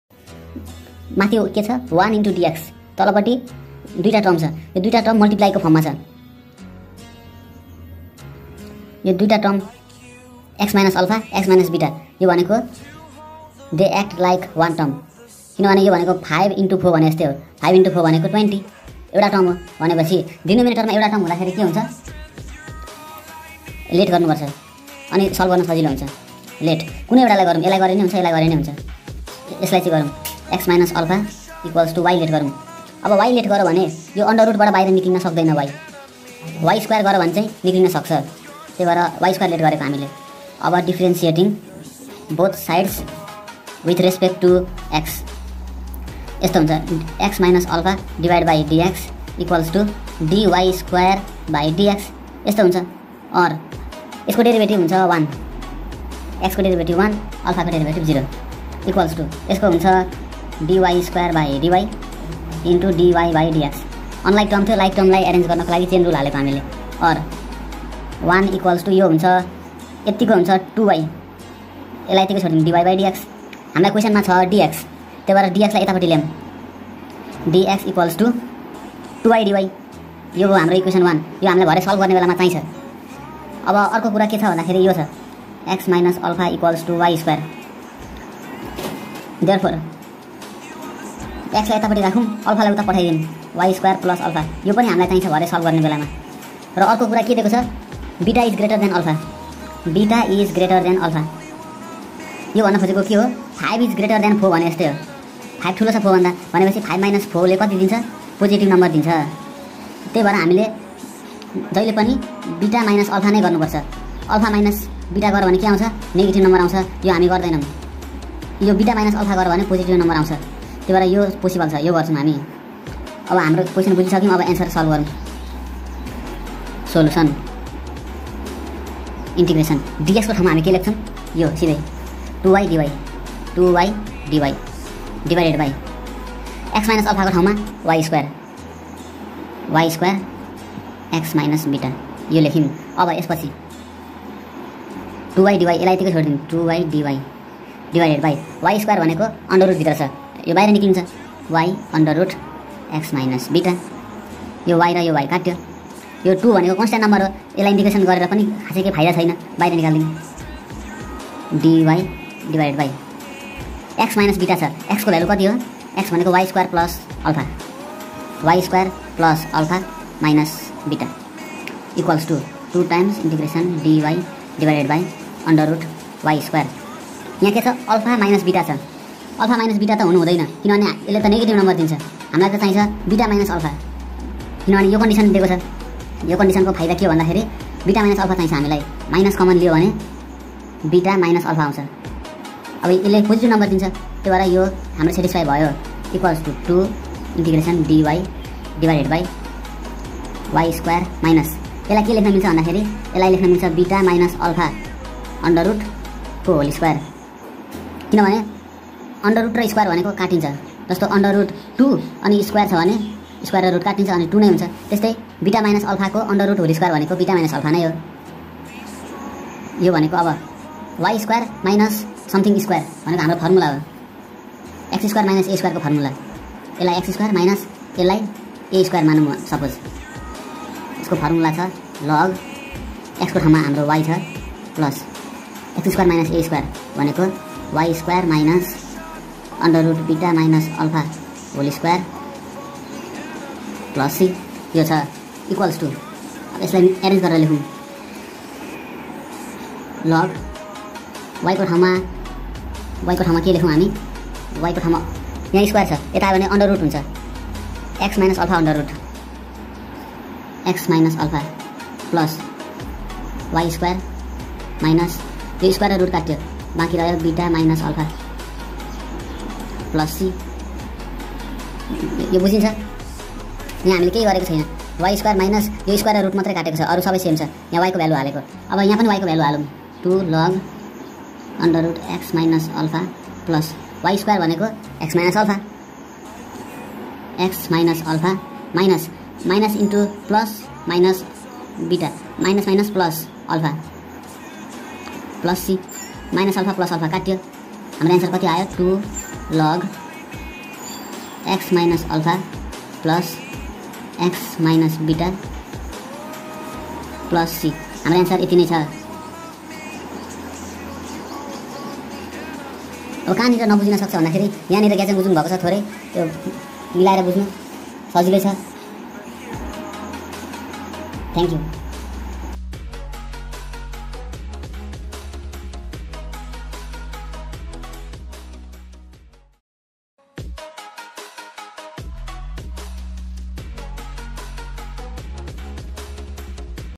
2. Integrate (β greater α) (xvi) ∫ dx/√((x–α)(x–β)), int dx/sqrt((x–alpha)(x–beta)) Antiderivative💥🌻
2. Integrate:
 (β greater α) (xvi) ∫ dx/√((x–α)(x–β)),
xvi) int dx/sqrt((x–alpha)(x–beta)) such that
(beta greater than alpha)
 Antiderivative Calculus MSI NEB 12 💥🌻

|| Unit 6 Calculus, Chapter 16 Antiderivative Class 12 Math
First Exercise NEB ||

Class-12, Unit 6 Chapter 16, Antiderivatives Calculus Maths Sukunda Publication. NEB . Easy solutions.

Mero Guarrantee 🏆

-  Krish 💎